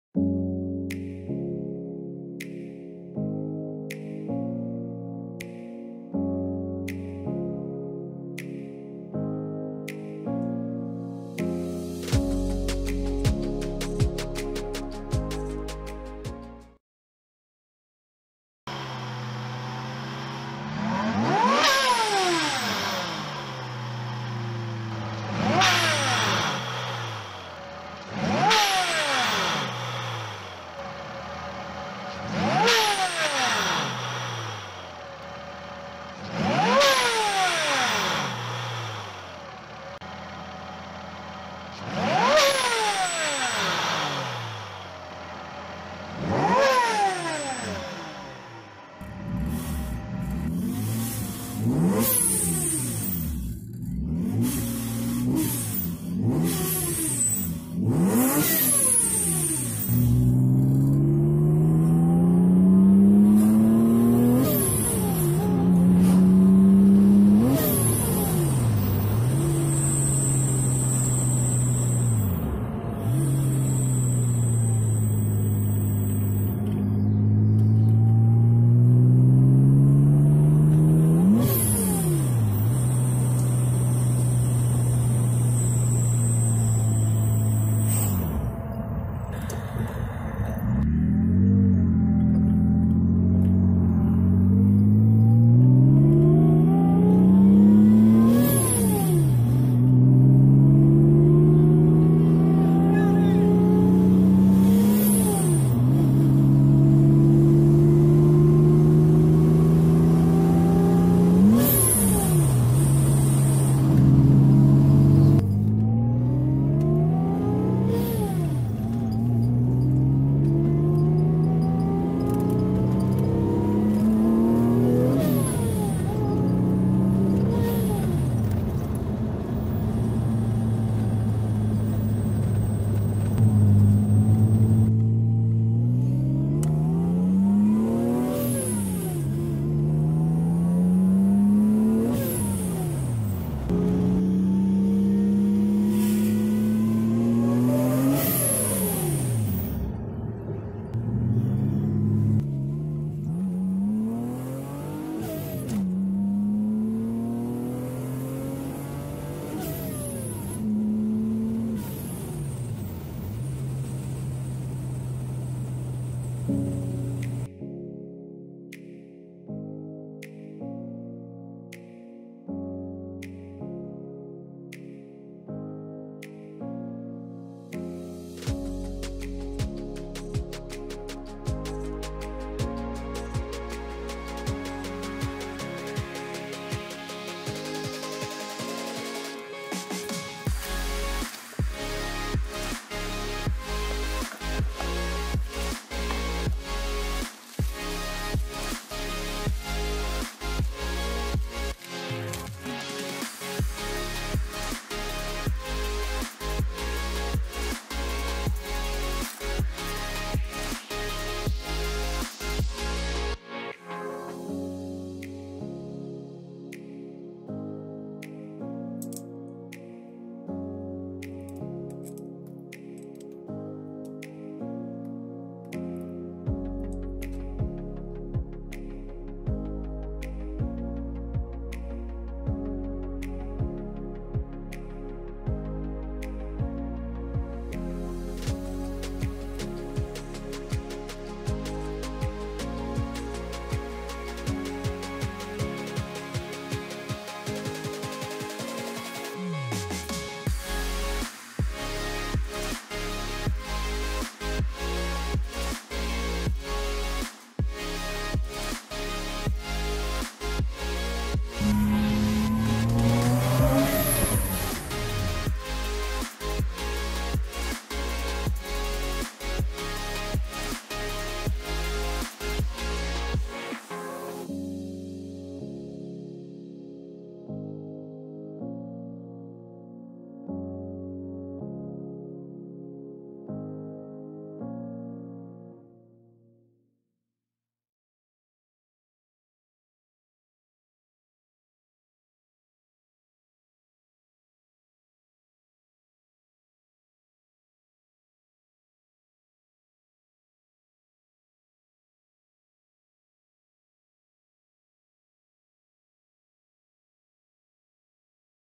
Greddy Type S BOV on a supercharger setup
Greddy Type S blow off valve sound demo, ebay find reveal and install. This is on a supercharger setup.